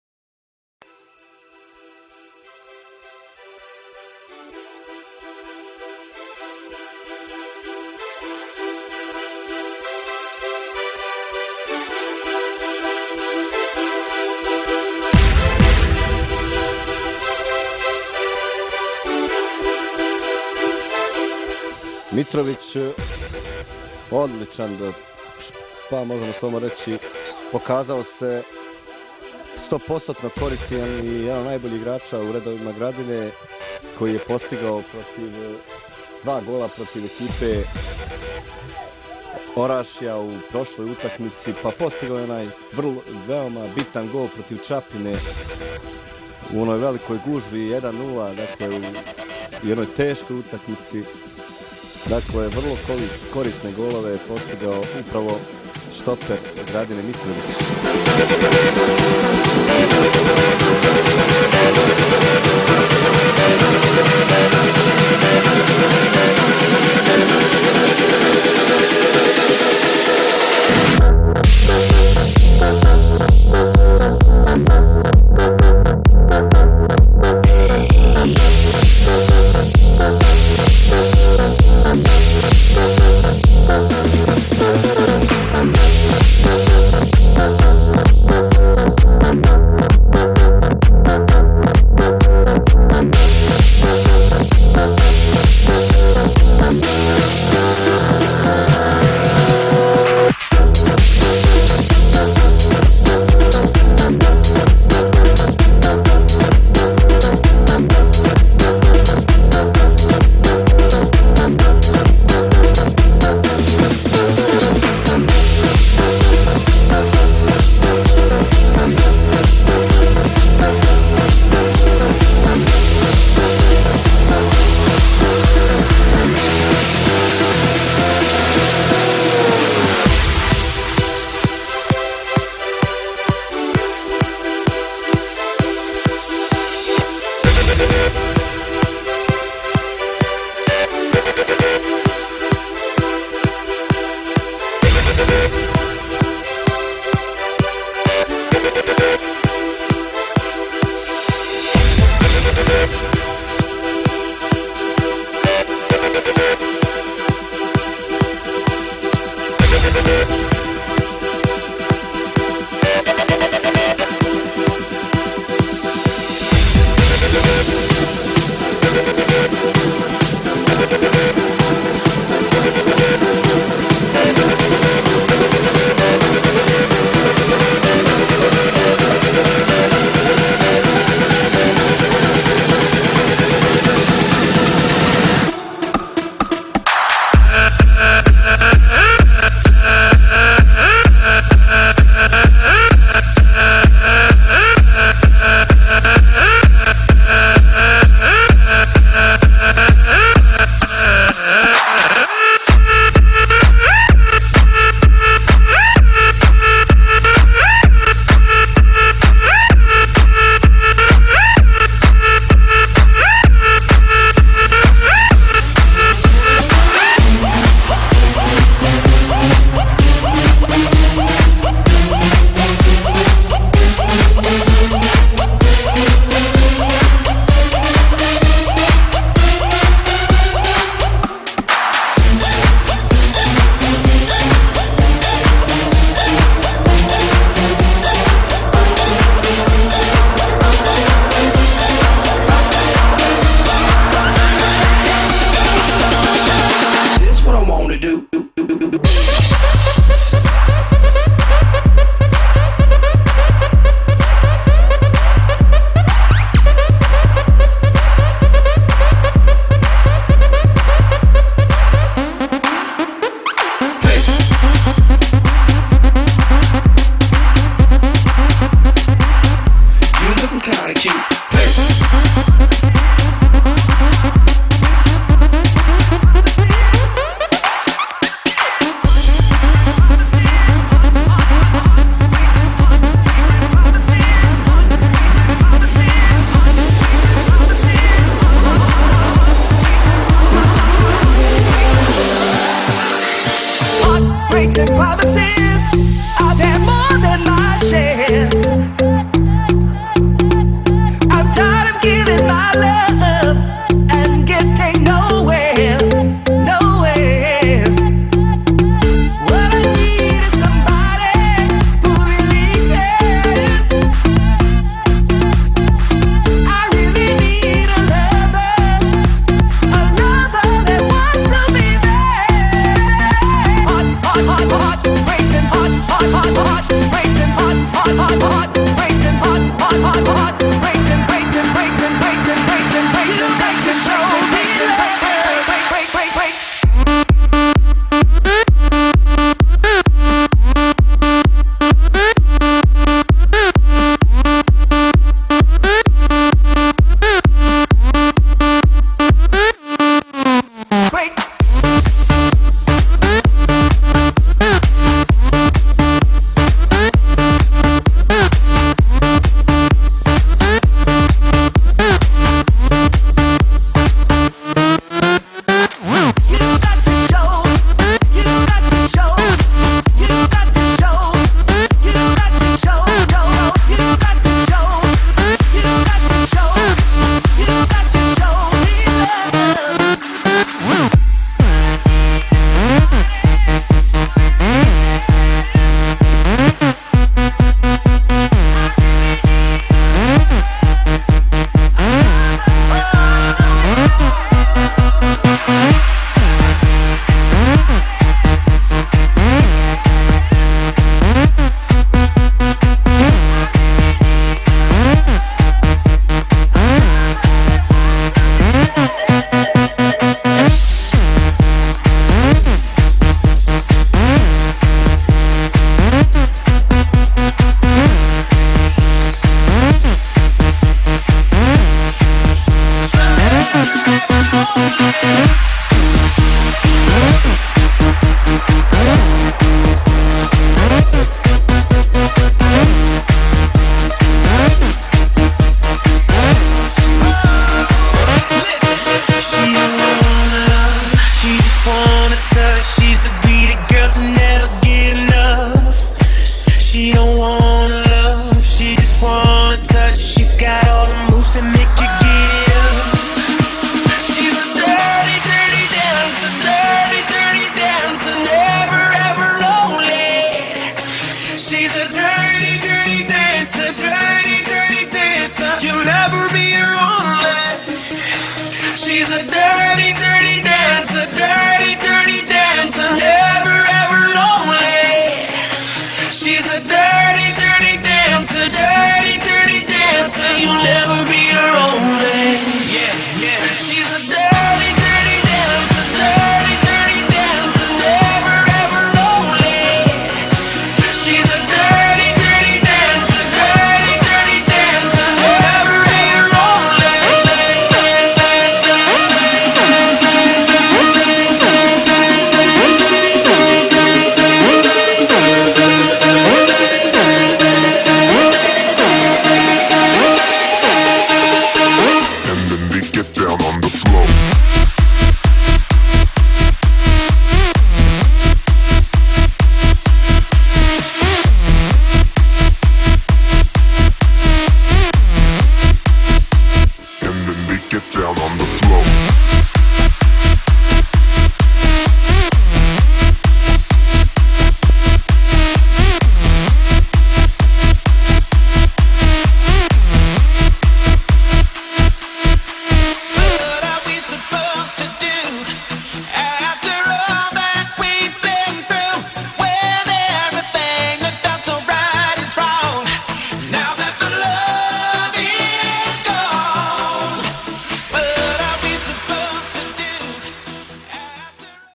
Mitrovic Darko - season 2011-2012 NK Gradina, FK Sutjeska
Darko Mitrovic FK Sutjeska Foča 2010 - VIDEO
3 goals, best scorer of the team